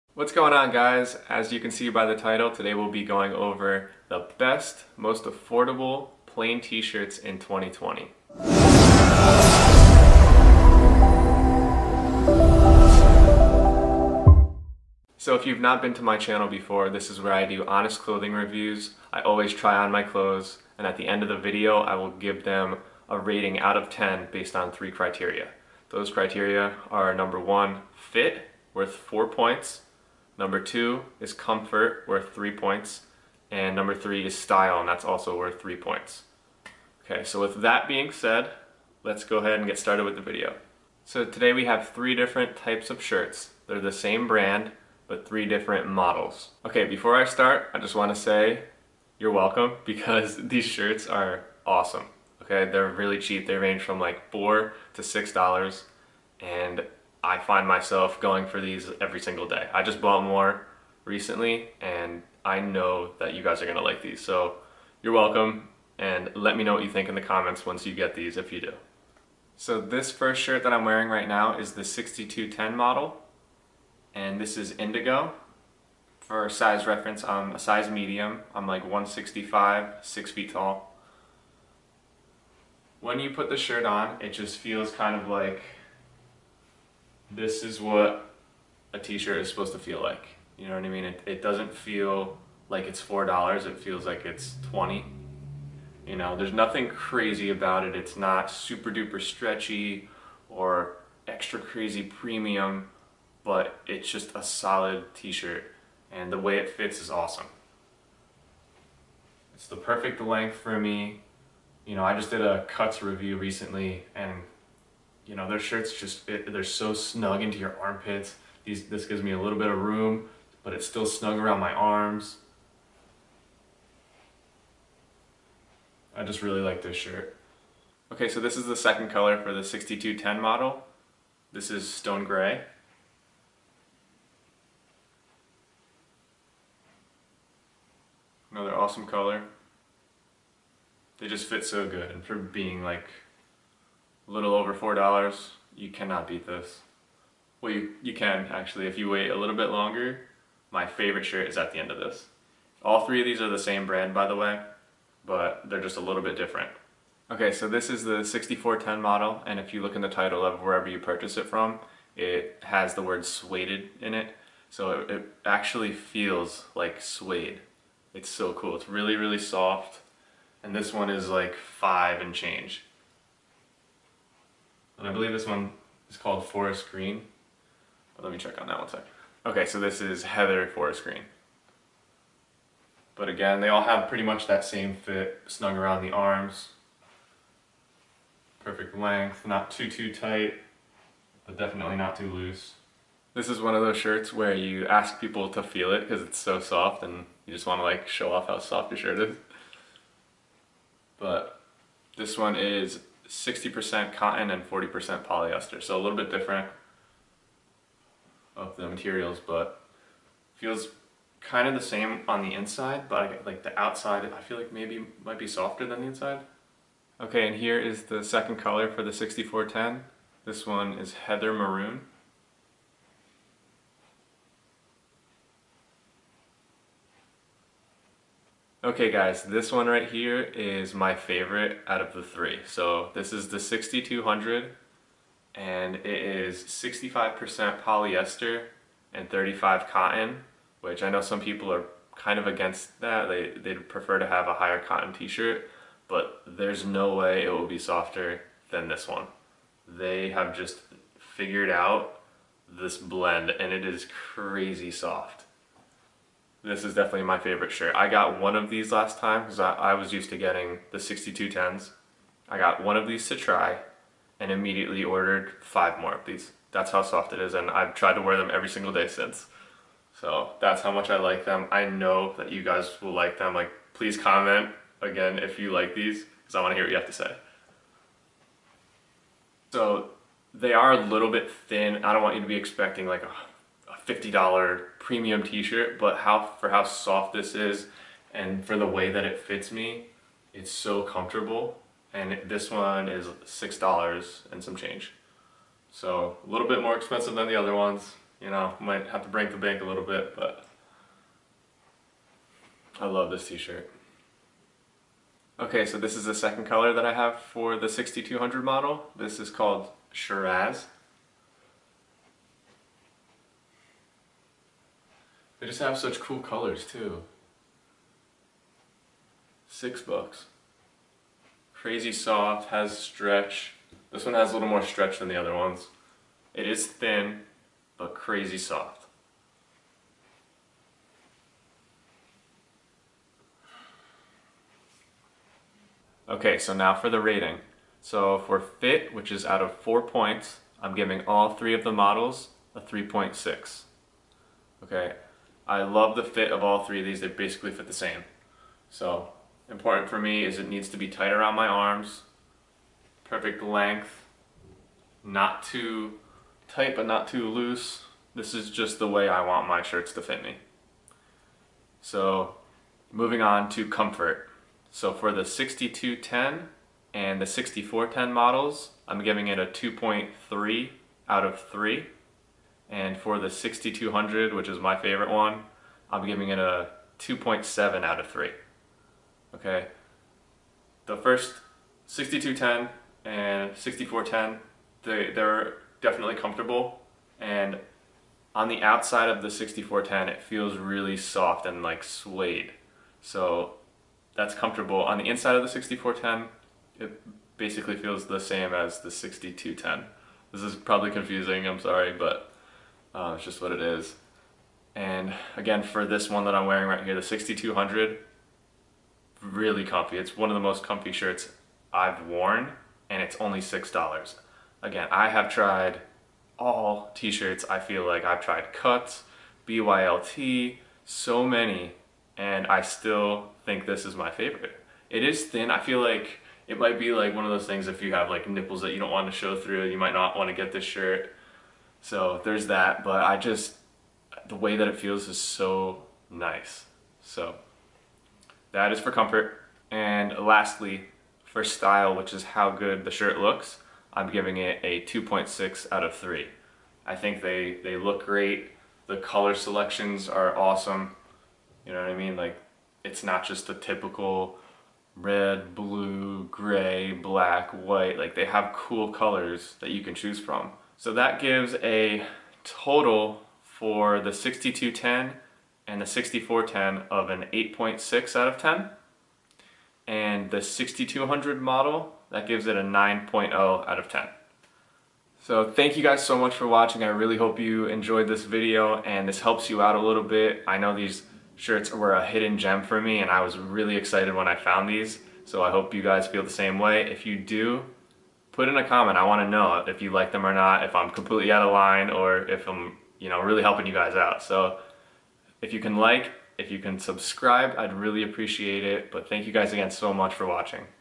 Best & Most Affordable Plain T Shirts
*** Links to get these shirts below ***

Next Level 6410 - Months after filming, this model is now my favorite

All of my clothing review videos will be scored out of 10 based on the three criteria below.

1. Fit - 4pts
2. Comfort - 3pts
3. Style (how it looks) 3pts

New videos will be posted every week.